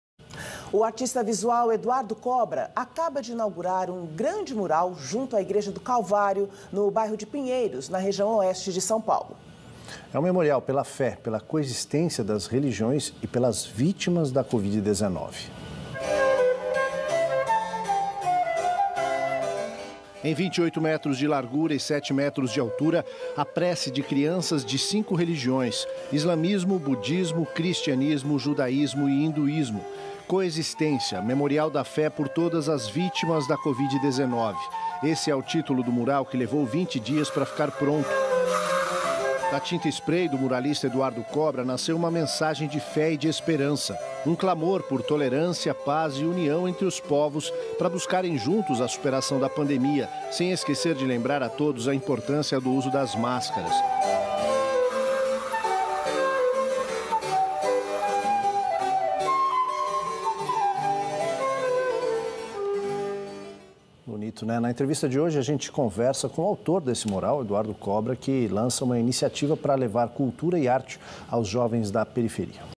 SP: Eduardo Kobra inaugura mural em Pinheiros
O "Memorial da Fé" homenageia vítimas da Covid-19 e preza respeito por todas as religiões